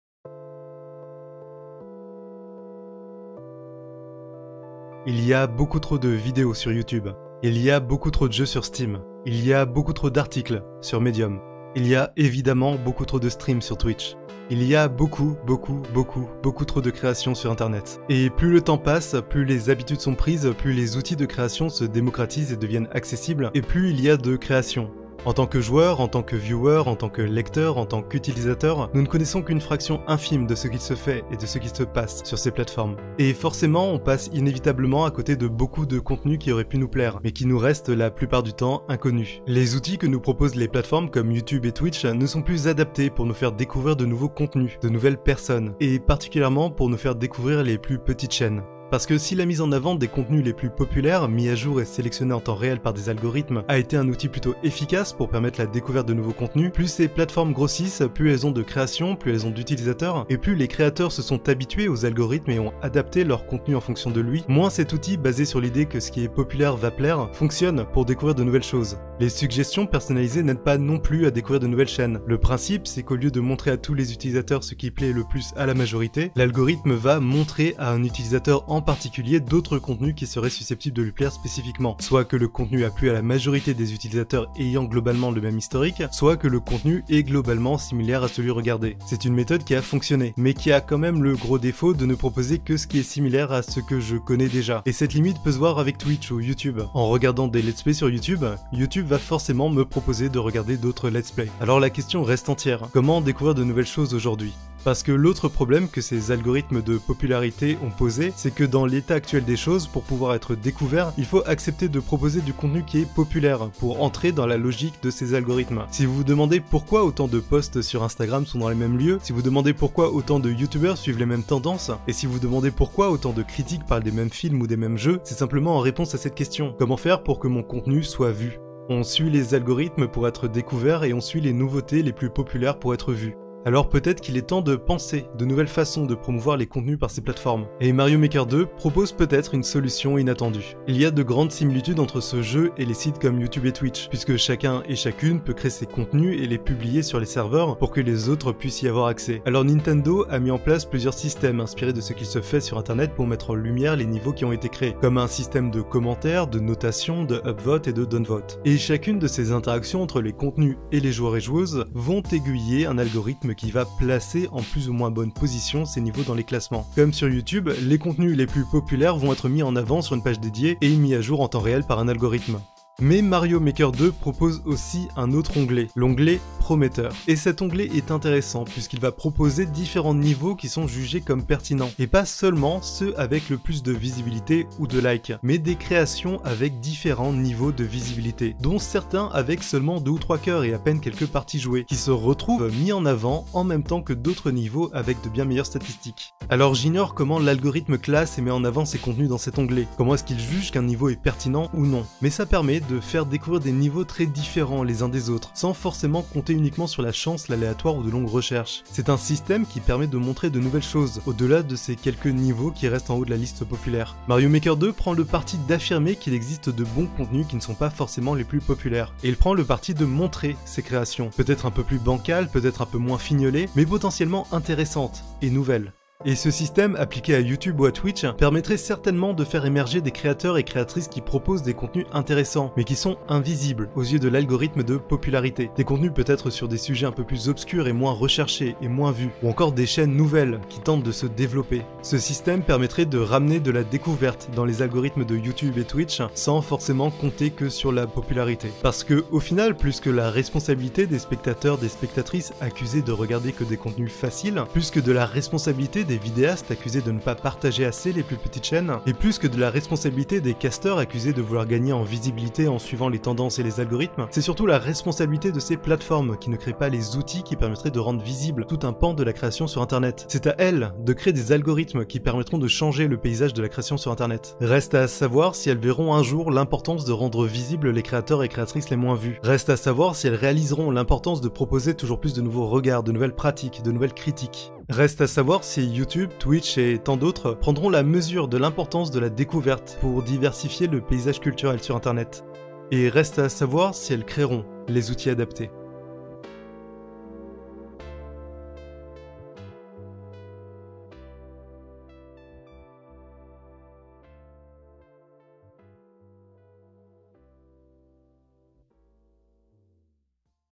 Ce que Youtube pourrait apprendre de Mario Maker 2
Et si Mario Maker 2 proposait une des meilleures idées pour réparer les Tendances sur Youtube ? + 7 chaines à découvrir dans la description !

Des récits et des interrogations sur les jeux indépendants et des découvertes de fan art avec Excursion Virtuelle

Des critiques et des tests de jeux vidéo, et des tutos pour Super Smash bros avec Beyond Two Minds

Et si vous voulez découvrir encore plus de ces petites chaines, le travail de curation de Youdeo est exceptionnel